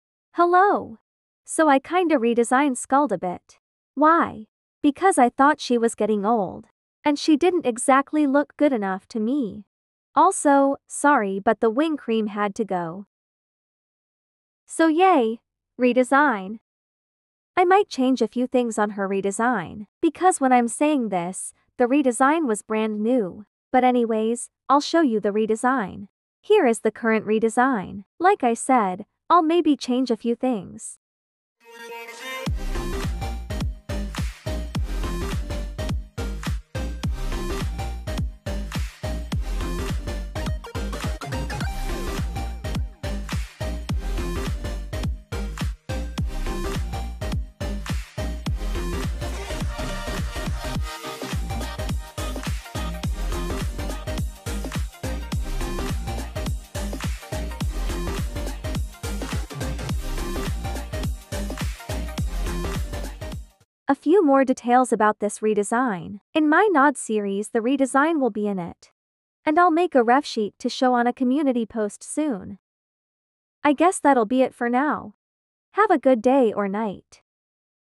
Skulled’s new redesign… || Read description for more
I will make a reference sheet eventually for the new design

Things metioned in the video:
Why did I make a redesign?
Skulled is a very old character of mine so I wanted to give her a redesign, I think the redesign is pretty good but I might change a few things on the redesign if it turns out that it doesn’t look very good.
The things removed…
Wing whites, I do have a story for the wing whites but its an old concept, the tail feathers were just for show but it looks kinda odd…like blood or a rat tail lol
Things added:
Darker colors, tail fade thing, dark legs/feet more colors to the head piece

That should be it…I think
I hope you guys like the redesign :)

I used the energetic female voice thing for the voice (on capcut)